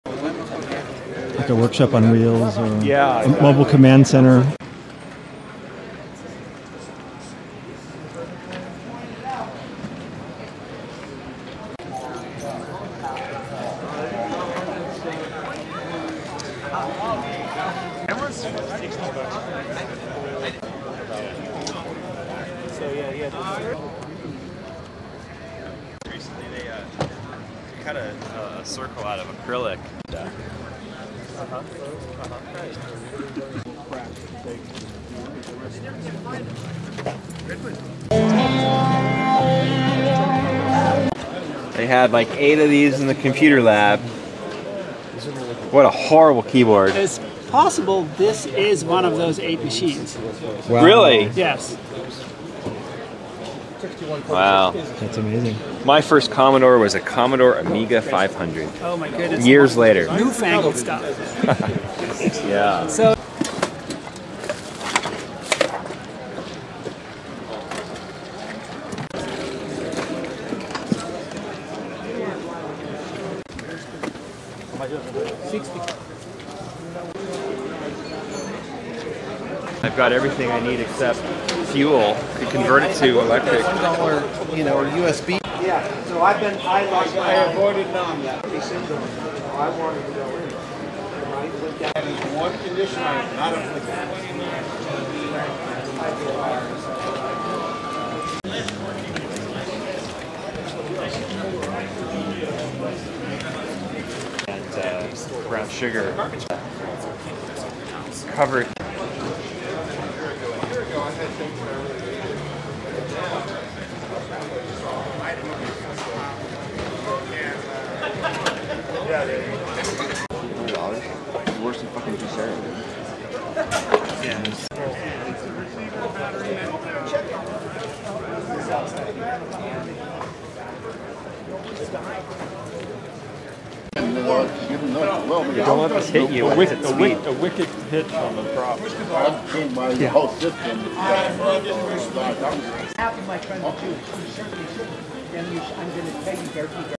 Electronics Flea Market | Cupertino California | March 10 2018
I went to the Electronics Flea Market with my brother on March 10 2018. The event took place at De Anza College in Cupertino California. At the time it was held on the second Saturday of the months March through September.

A new season of Electronics Flea Markets begins March 10 2024 at West Valley College in Saratoga California.

Original Footage:
- 2160p 59.95 FPS
- Taken with a Canon GX10 Camcorder

NO VIDEO EDITOR WAS USED TO MAKE THIS VIDEO!!

Instead I used the Video Remixer tool from my open source AI-based video renovation suite for Windows/Linux and NVIDIA GPUs: